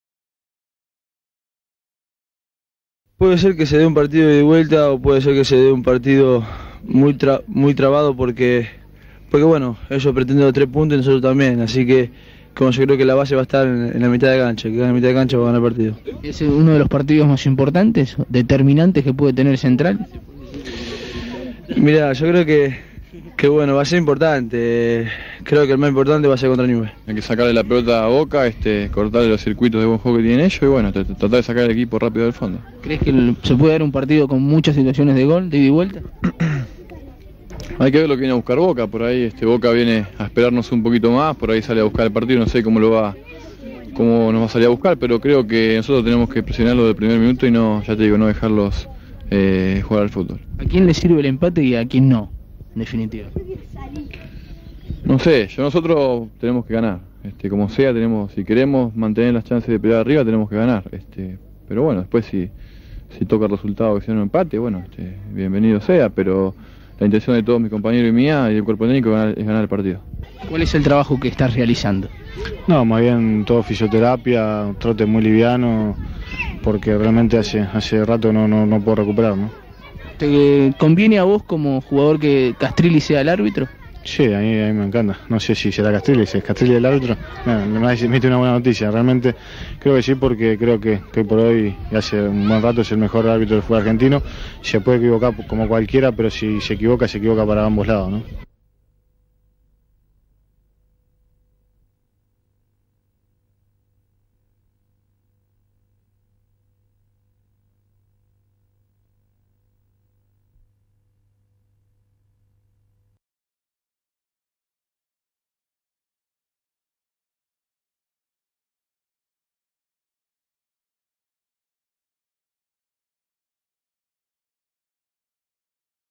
Reportaje a Gaston Sessa - Fernando Da Silva - Rosario Central 1997
Entrena el plantel profesional de Rosario Central con vistas al partido con Boca Juniors por la Fecha 12 del Torneo Clausura. Nota a Gaston Alejandro Sessa, Ruben Fernando Da Silva y otro jugador.
Fecha: 6/11/1997
Duración: 1 minuto 50 segundos
Código: BG-0461